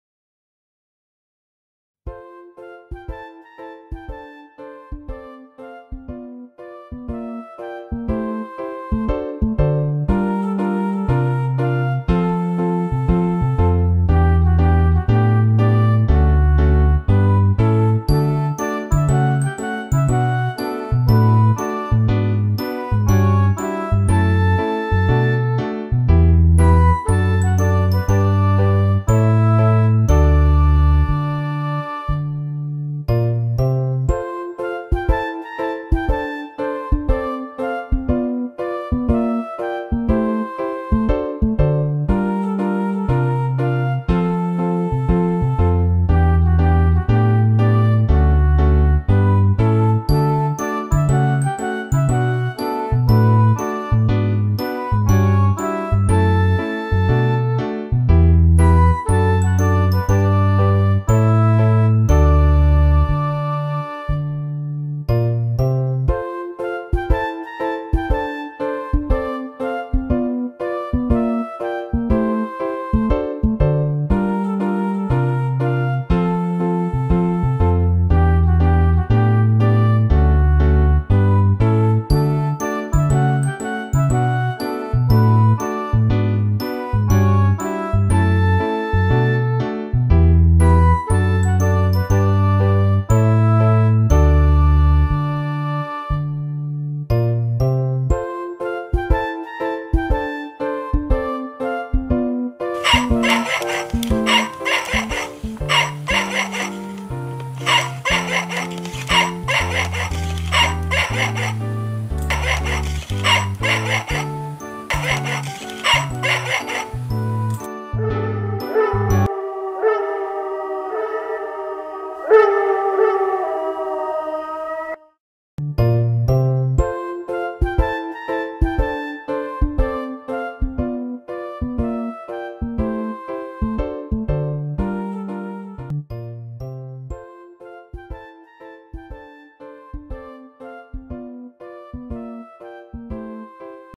Mr Pencil and the Sharp End Pup horror movie
A not so nice ending for Mr Pencil with the Sharp End Pup pencil sharpener